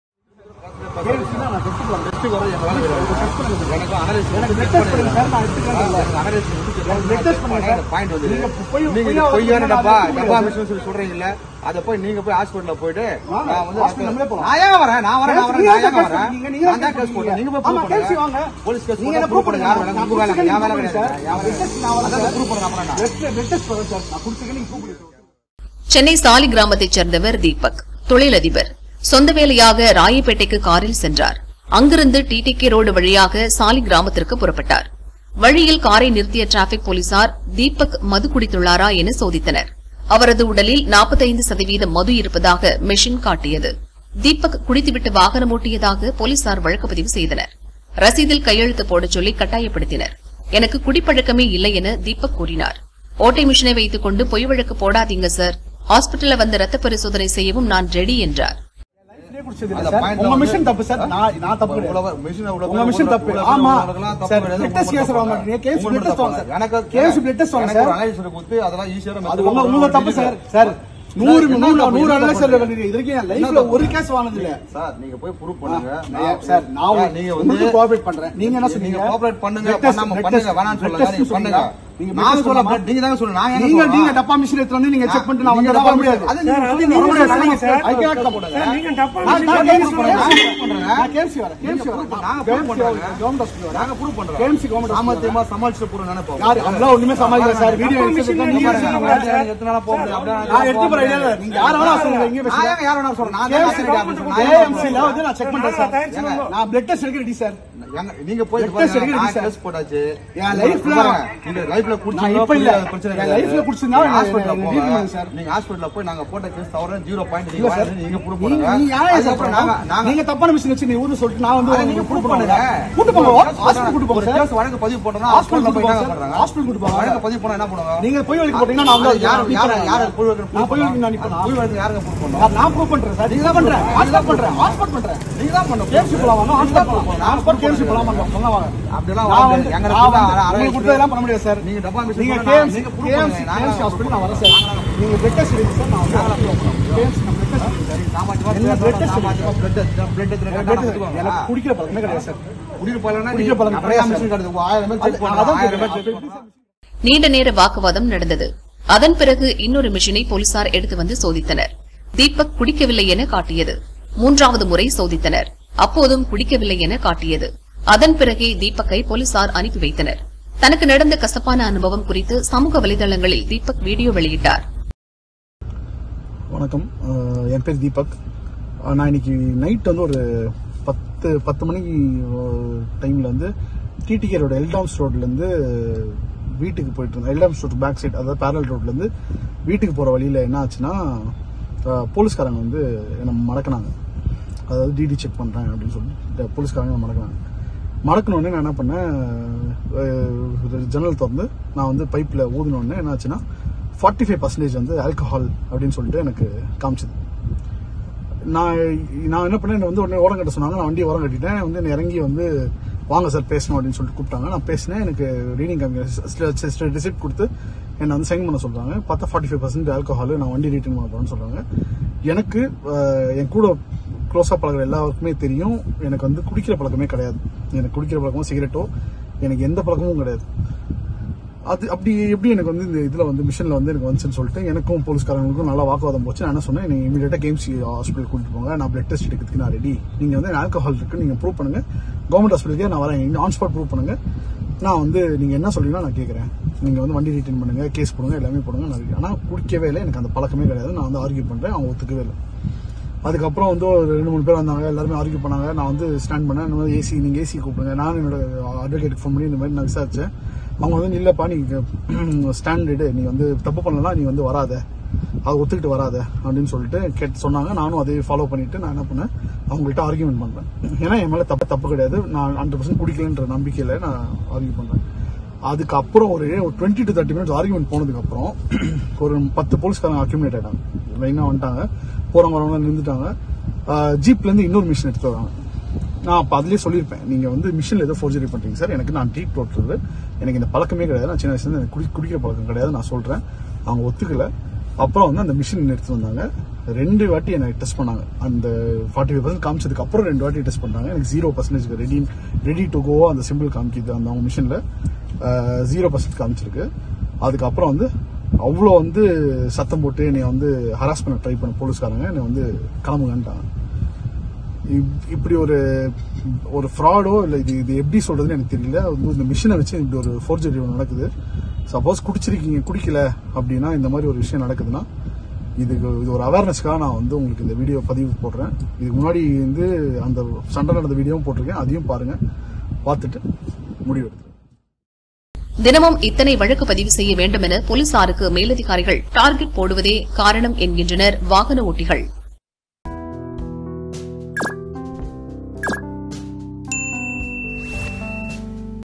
போலீசுடன் தொழிலதிபர் வாக்குவாதம் | Drunk and Drive | Chennai | Dinamalar
டப்பா மெஷின்ல டெஸ்ட் பண்ணி குடிச்சேன்னு சொல்றது அநியாயங்க!| போலீசுடன் தொழிலதிபர் வாக்குவாதம் | Drunk and Drive | Chennai | Dinamalar#DrunkandDrive